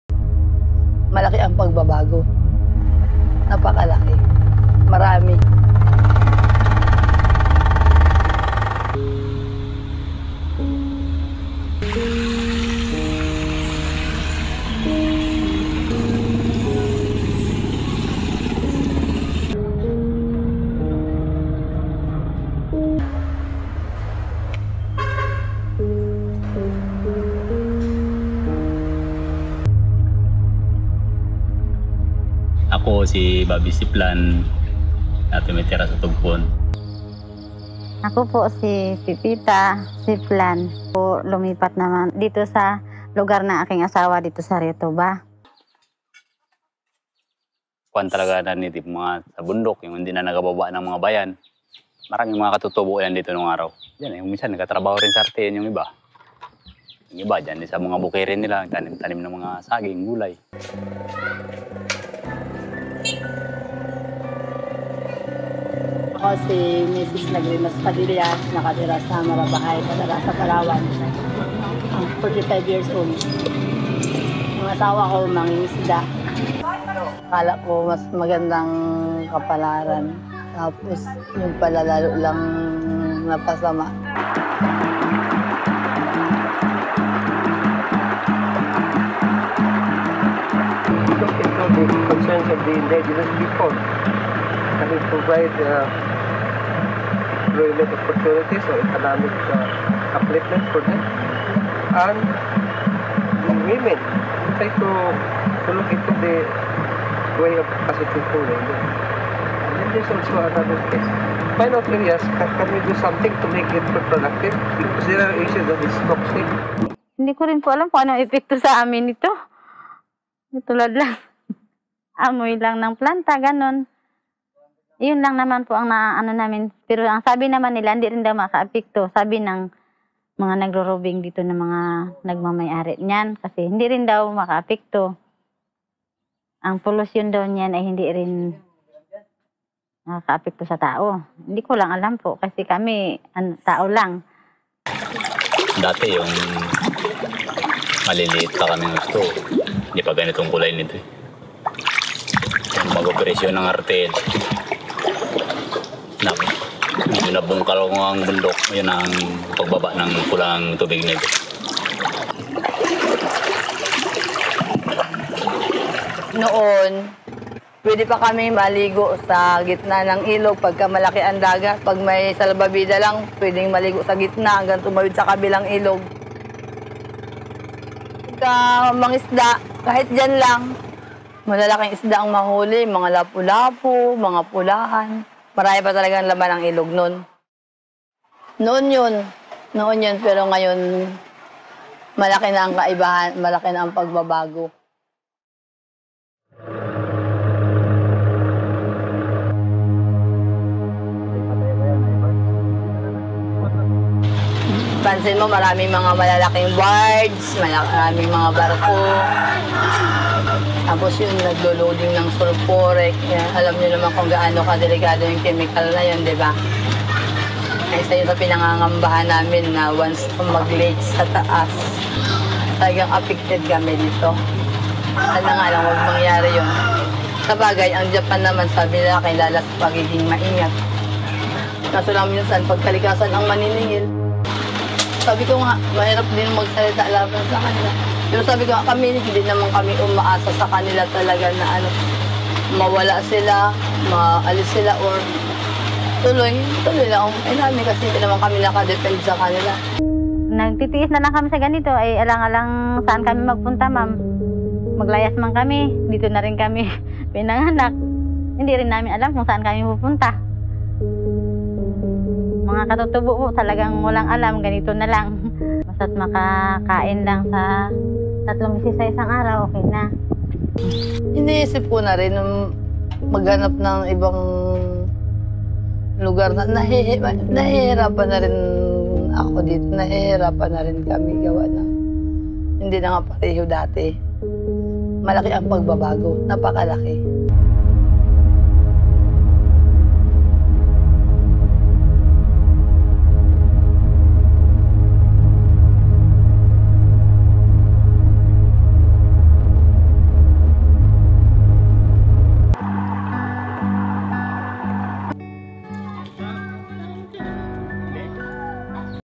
RIOTUBA NICKLE MINING CORPORATION
Rio Tuba in Palawan is the site of the one of the biggest nickel open pit mine in the Philippines. It is also home to a once thriving fishing community along the Togpon River. Bobby Siplan, fisherman, and Lagrimas Padilla, fish vendor, are both members of this community. Two lives whose daily struggle to make a living ebb and flow with the changes wrought upon the river by the mining industry. The mine promises a harvest of bounty. Bobby and Lagrimas think otherwise. This is their story.